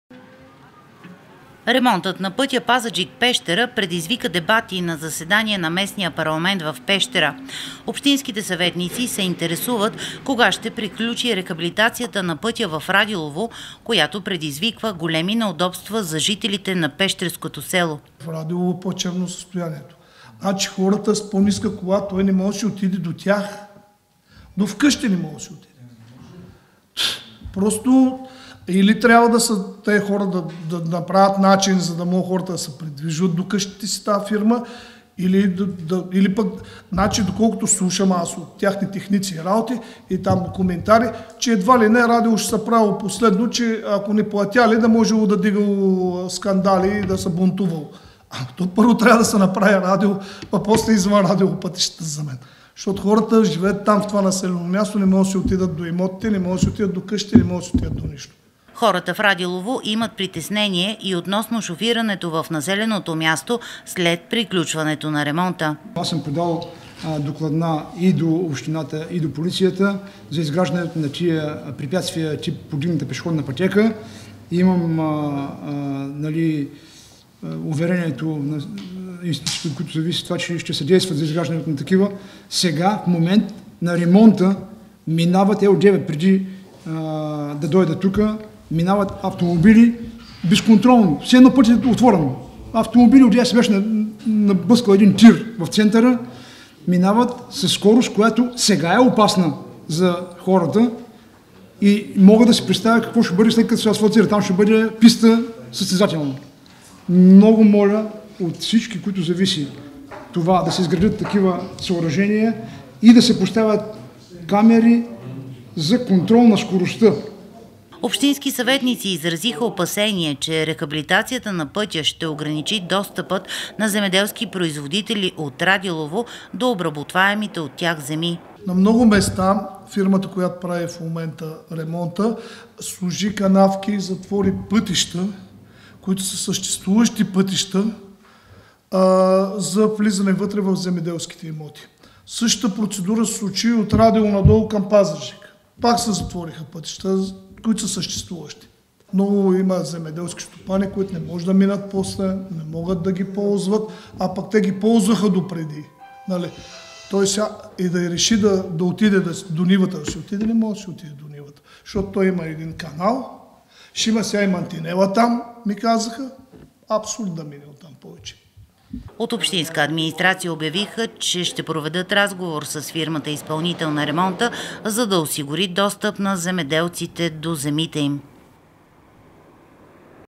Ремонтът на пътя в Радилово отново бе акцент на заседание на ОбС Пещера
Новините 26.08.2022